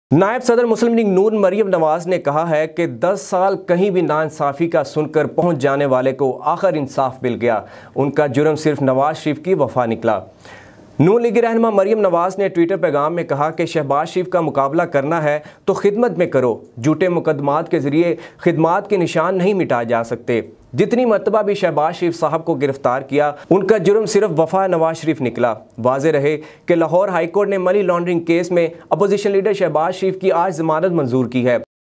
شہباز شریف کا مقابلہ کرنا ہے تو خدمت میں کرو۔ مریم نواز
لیگی رہنما مریم نواز کا کہنا تھا کہ دس سال کہیں بھی ناانصافی کا سن کر پہنچ جانے والے کو آخر انصاف مل گیا، شہباز شریف کو جب بھی گرفتارکیا گیا تو ان کا جرم صرف  نواز شریف سے وفا نکلا۔